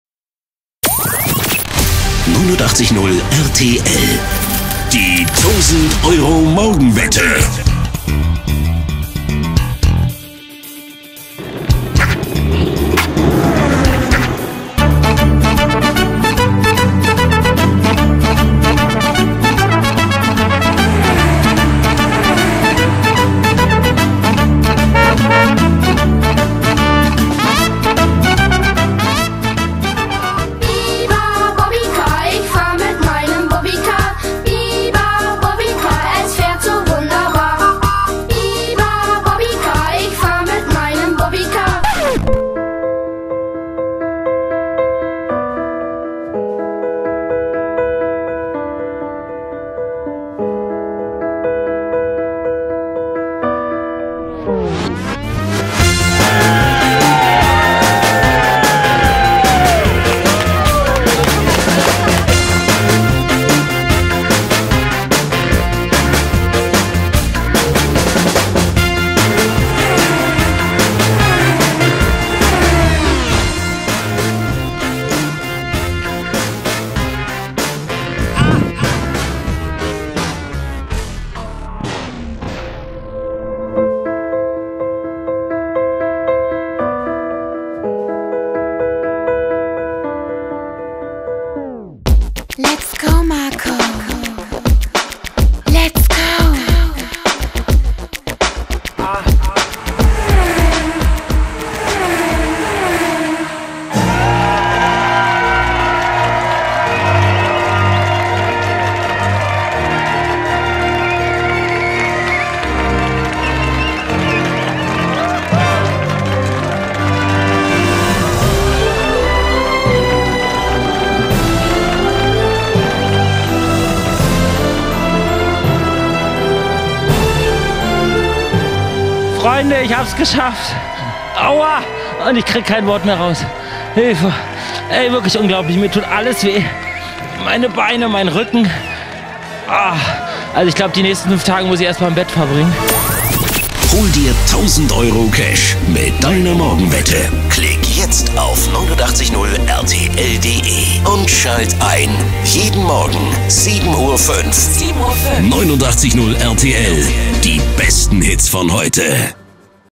Die 89.0 RTL Bobby-Car-Wette mit Marco
Olli aus Hannover wettet, dass Marco es nicht schafft, in einem Bobby-Car ganze 89 km um den Maschsee zu fahren. Bis zum Wochenende hat er dafür Zeit! Ob Marco das schafft?